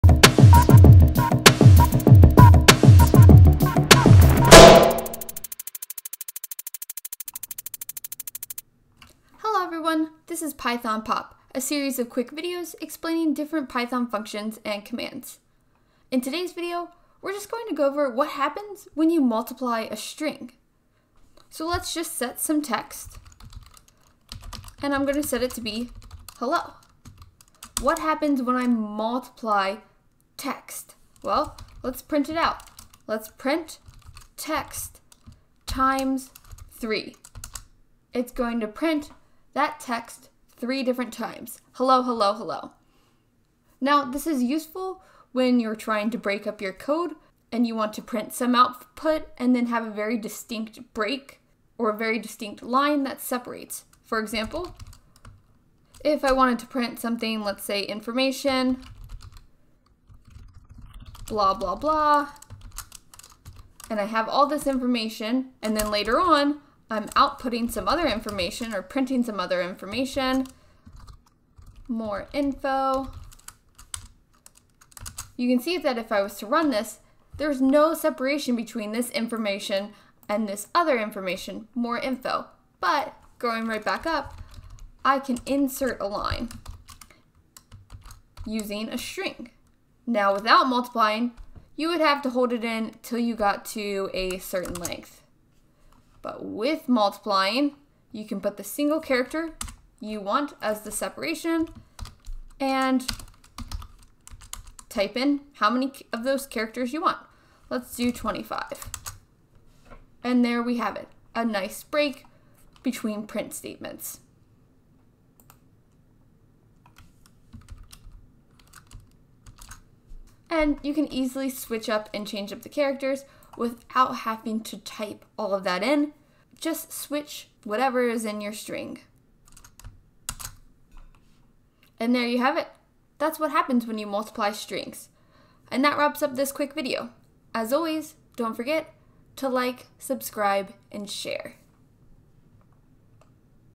Python Pop: Multiplying Strings
Python Pop is a series of quick videos explaining different functions and commands from the Python language. 💻 Have you ever wondered what happens when you 'multiply' a string? Check it out in this video and discover why it is so useful!

💛 Happy learning! 💙